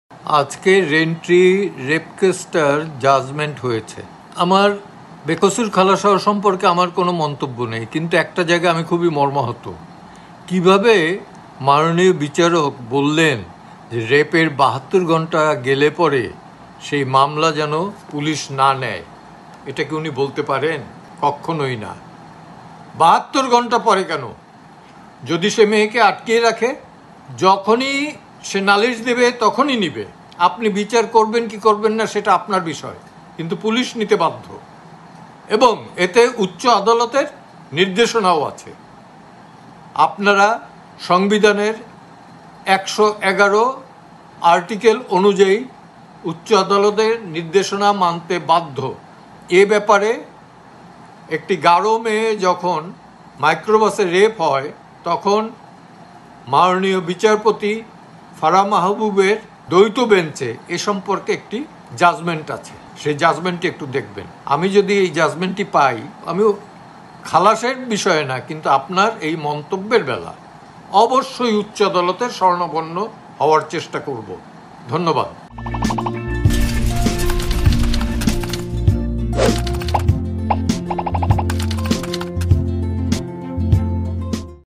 ধর্ষণের ৭২ ঘণ্টা পর মামলা না নেওয়ার ‘পরামর্শে’ ক্ষুব্ধ আইনজীবী || Jago News
ধর্ষণের ৭২ ঘণ্টা পর মামলা না নেওয়ার ‘পরামর্শে’ ক্ষুব্ধ আইনজীবী